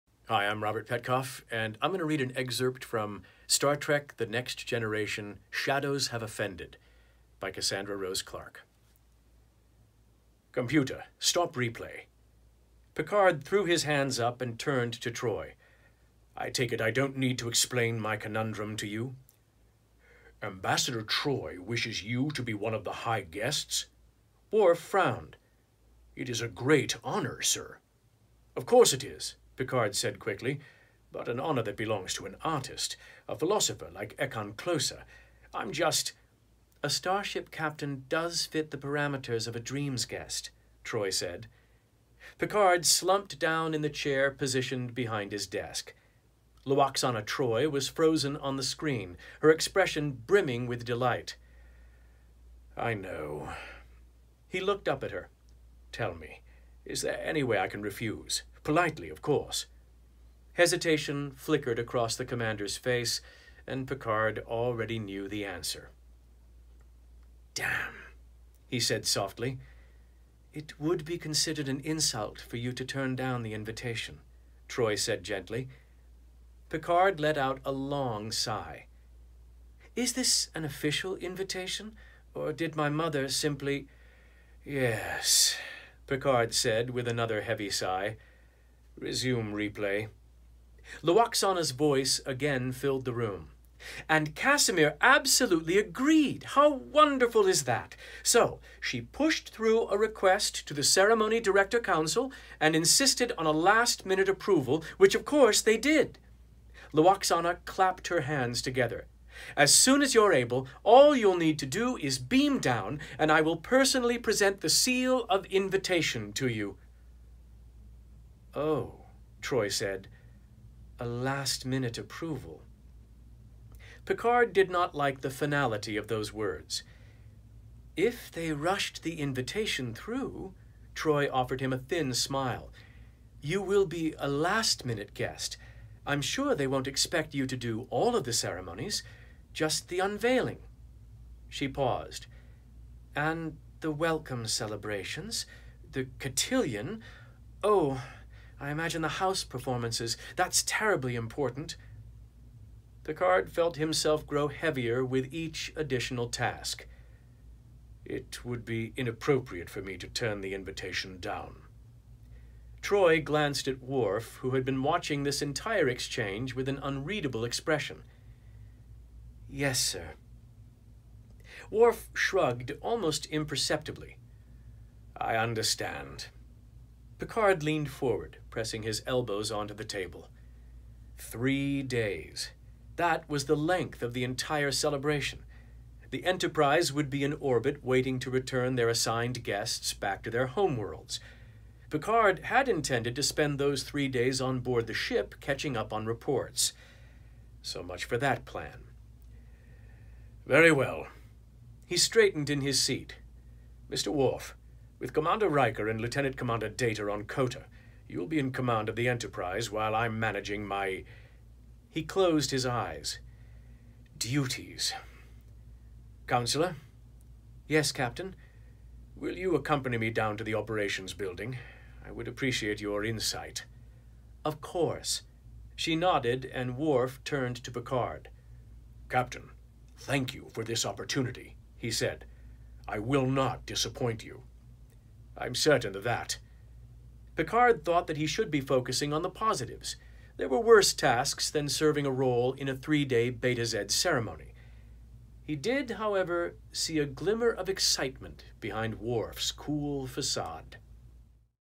Robert Petkoff narrates SHADOWS HAVE OFFENDED by Cassandra Rose Clarke
Watch Robert Petkoff read an excerpt from SHADOWS HAVE OFFENDED by Cassandra Rose Clarke.